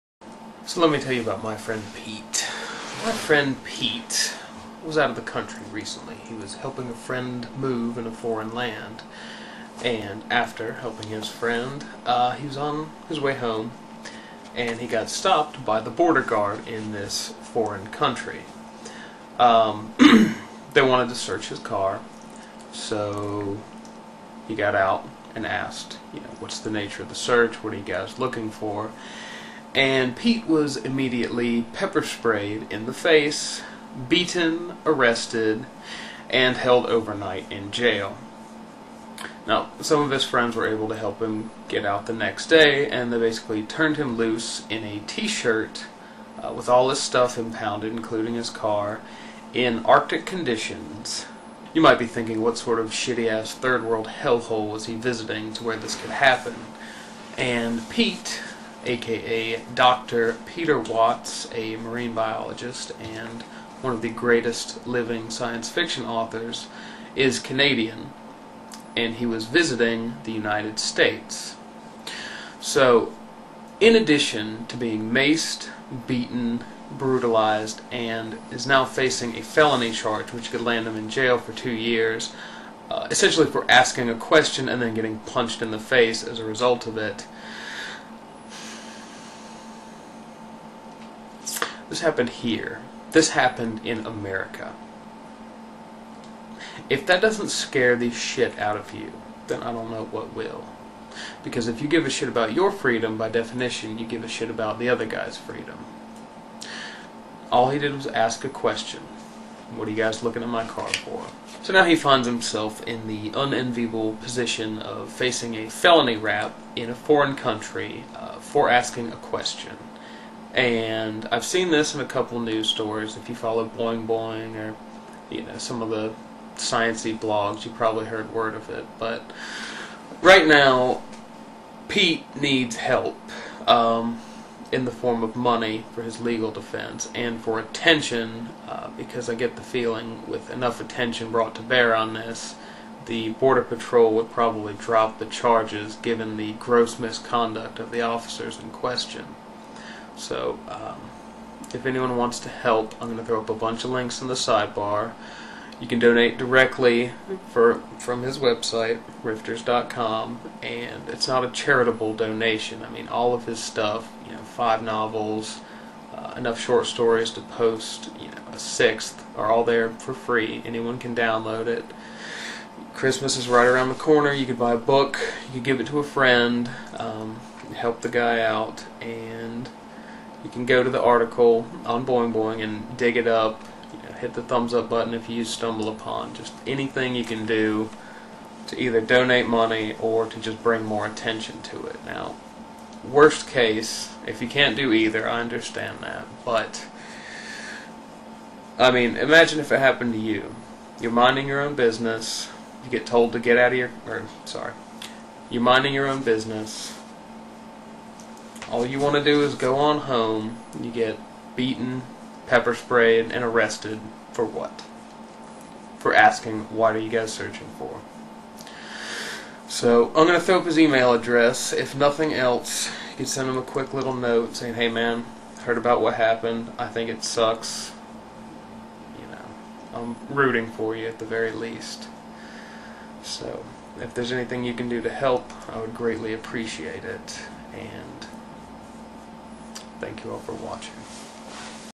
a grave injustice.
So Peter Watts was beaten and arrested.

Tags:  Peter Watts  Peter  Watts  Blindsight  Rifters  rifters.com  cory doctorow  science  science fiction  sci-fi  a grave injustice  keippernicus